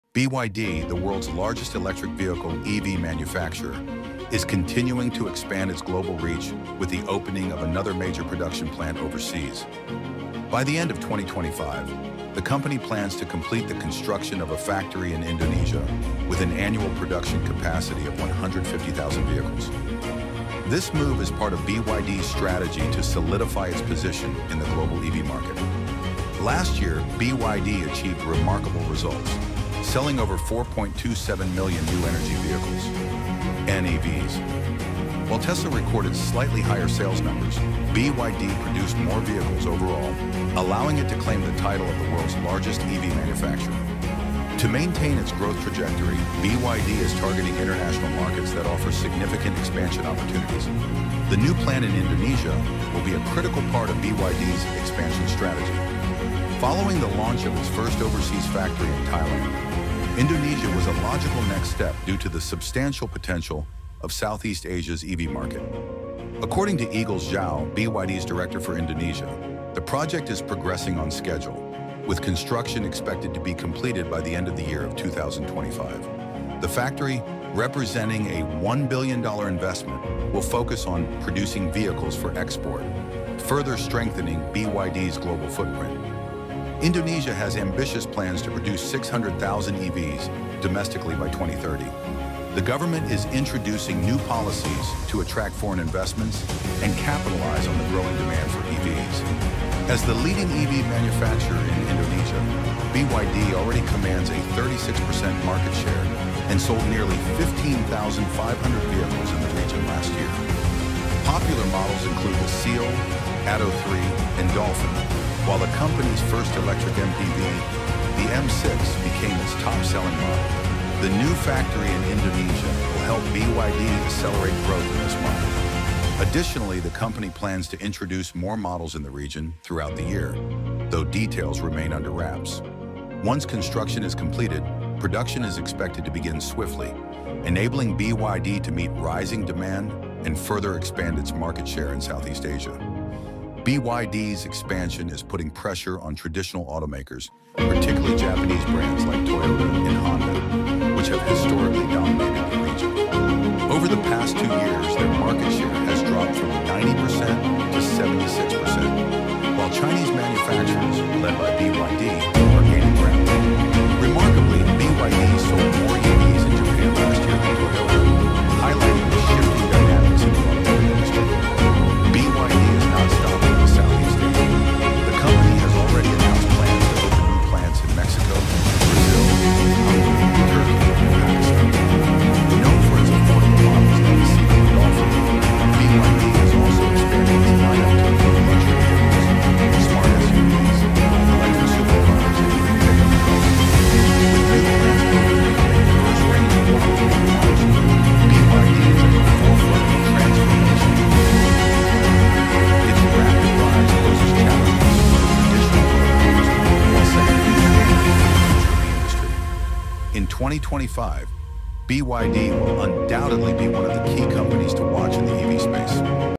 BYD’s Huge New Factory in Indonesia
BYD, the world's largest electric vehicle manufacturer, is taking a bold step in expanding its global footprint. With a $1 billion investment, the company is building a massive new EV production facility in Indonesia, capable of producing 150,000 vehicles annually. This video explores BYD’s ambitious plans, their recent successes in the global EV market, and how this new plant fits into Indonesia's vision of producing 600,000 EVs domestically by 2030.

Discover how BYD is reshaping the EV industry, challenging traditional automakers, and setting the stage for a cleaner, more sustainable future.